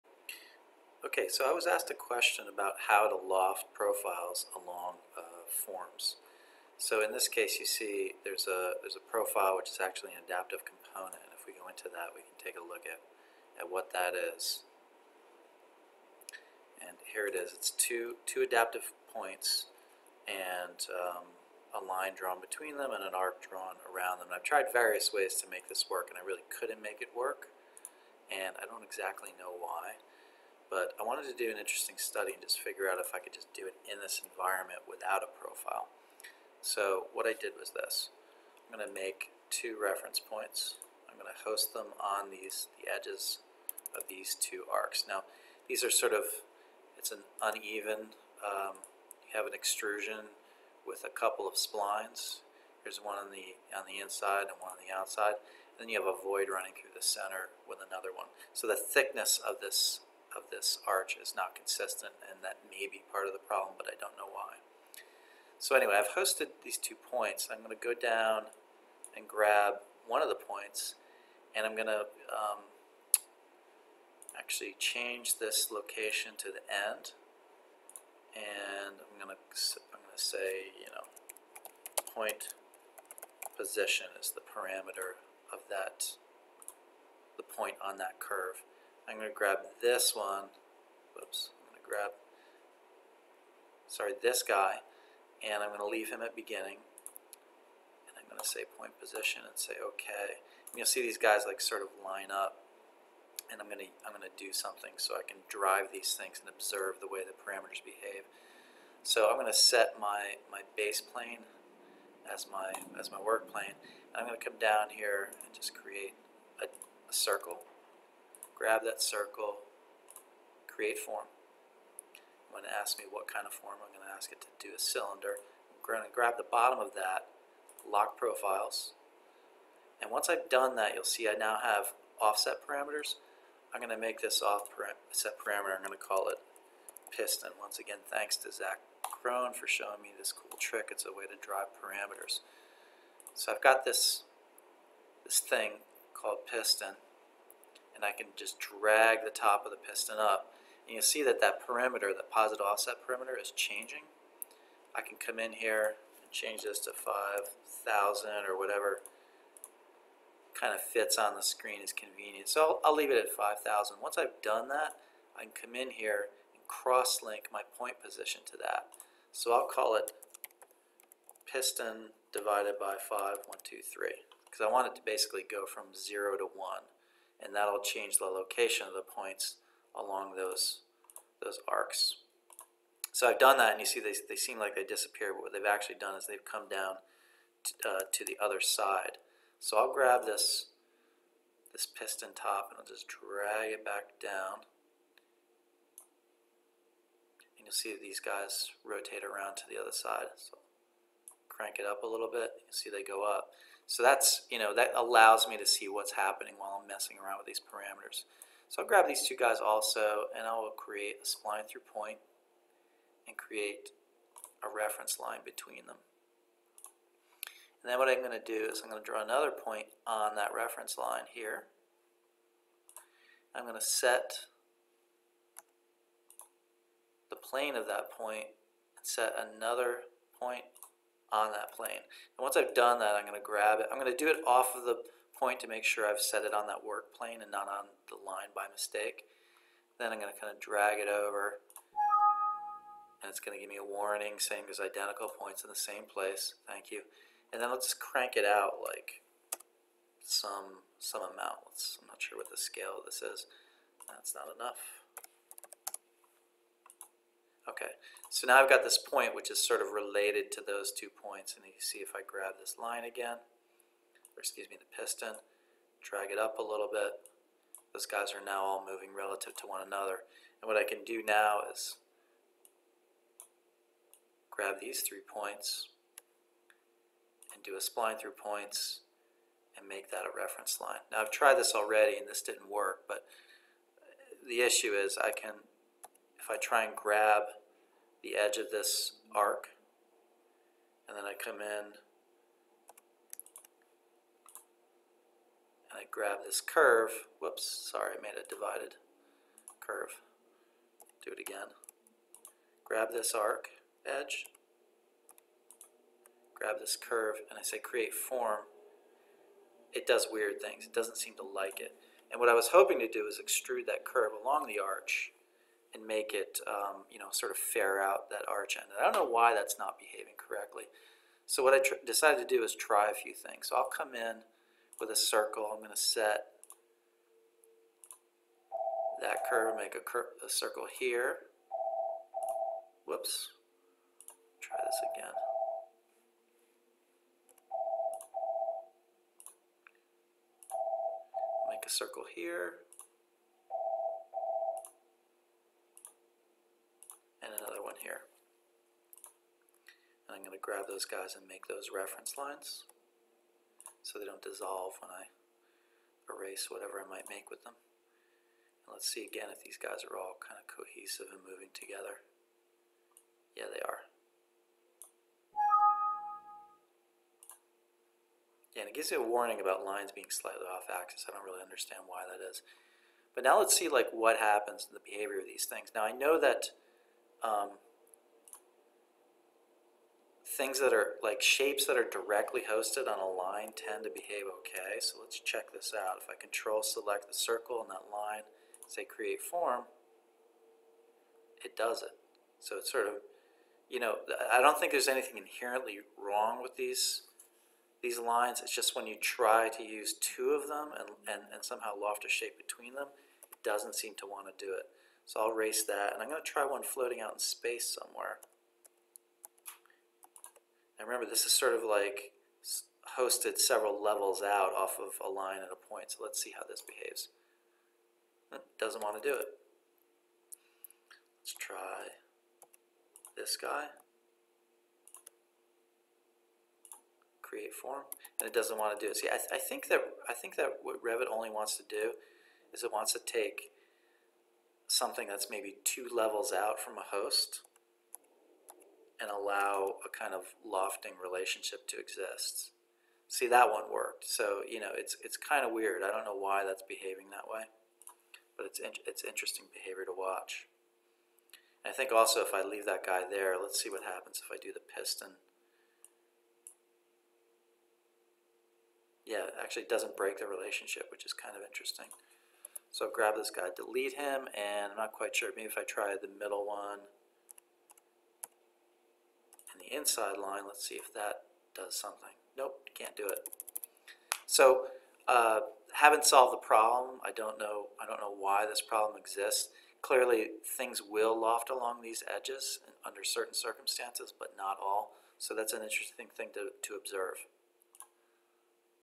Revit Lofting Profiles Along Form Edges - Success and Failure
Asked a question about whether one can loft a curve along two other curves, I tried and failed. However, I observed interesting behavior while trying. I suspect that lofting a profile along a curve  in this environment only works with one host curve but hopefully someone can prove that wrong.